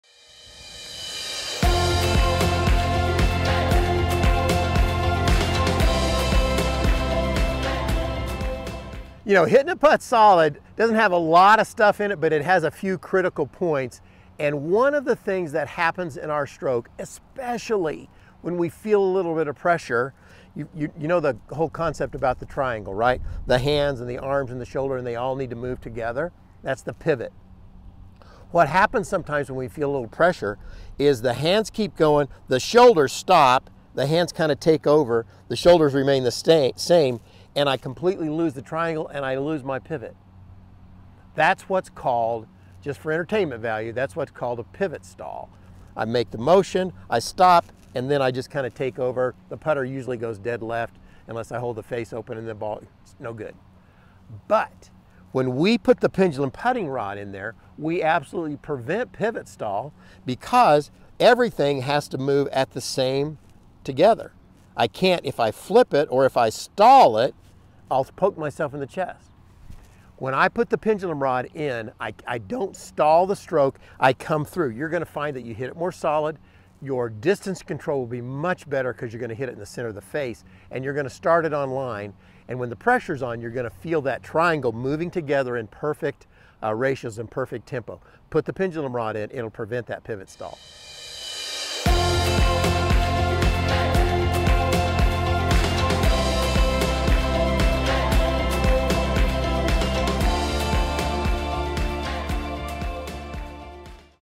Pivot Stall? Nope
Pivot Stall is probably what happens when you start missing the sweet spot with your putter.

Your “putting triangle” breaks down, the putter flips or is stuck open, and it is almost impossible to return the putter face squarely to the back of the ball.

Take a look at the Pendulum Putting Rod in action and you will see the "fix to the stall"!